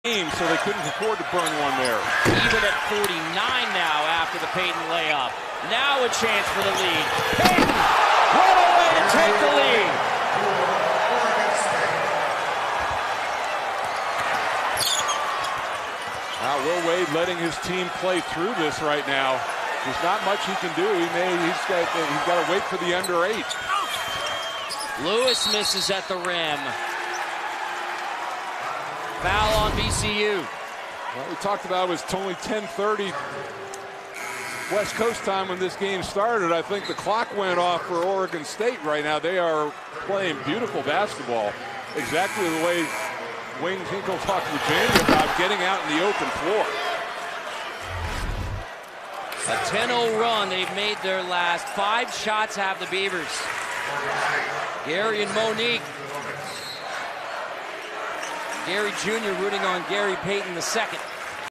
VCU vs. Oregon State: Gary Payton II's highlight-reel dunk
In Oklahoma City, Oregon State's Gary Payton II throws it down bringing the Beavers back into contention against VCU.